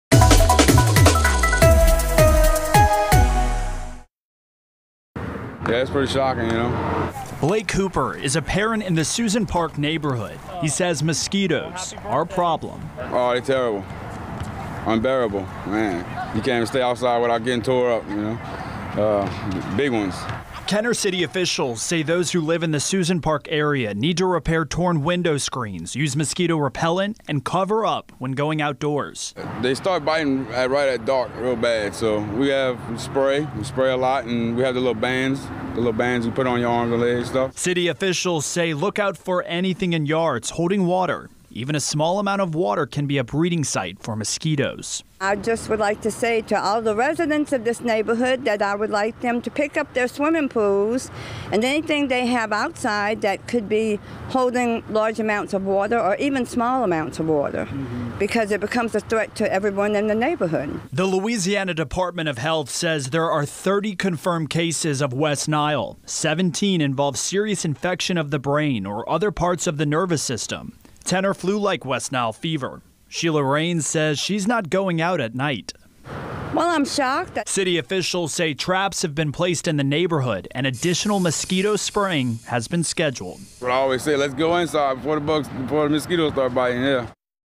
Parents concerned after West Nile confirmed in Kenner neighborhood
Kenner officials say West Nile has been detected in the Susan Park area.